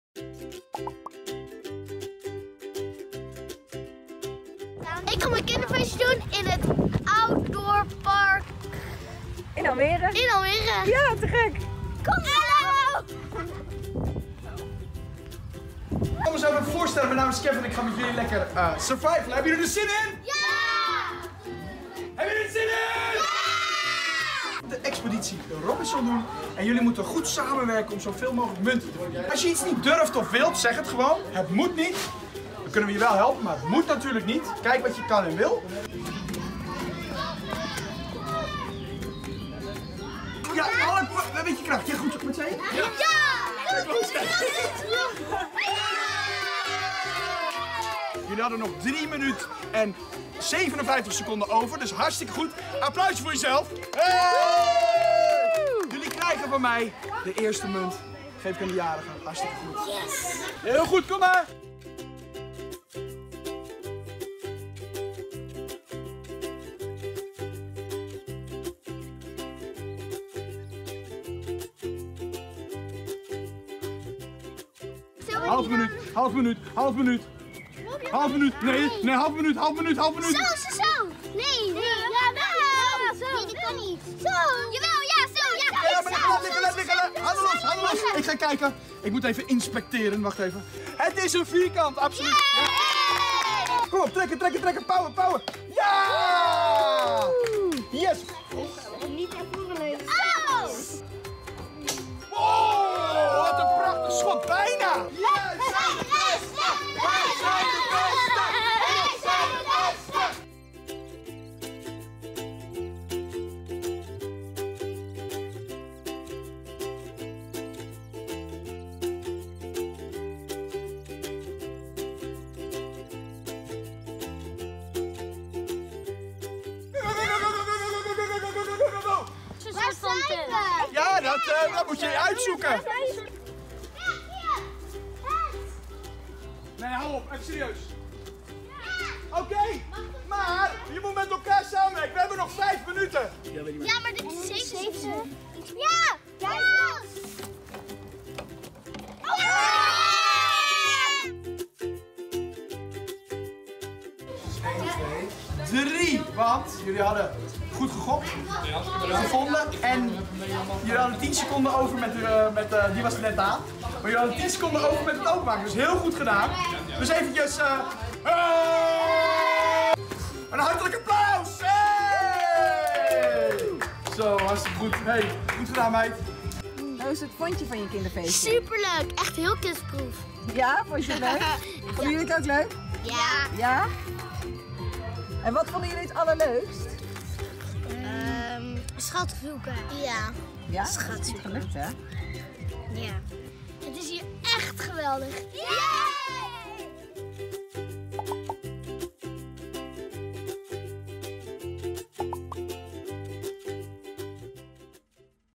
★ Kinderfeestje bij Outdoorpark SEC Almere - Kidsproofreporter Loïs
Onze eigen Kidsproofreporter Loïs mocht een te gek Expeditie Robinson kinderfeestje vieren bij outdoorpark SEC.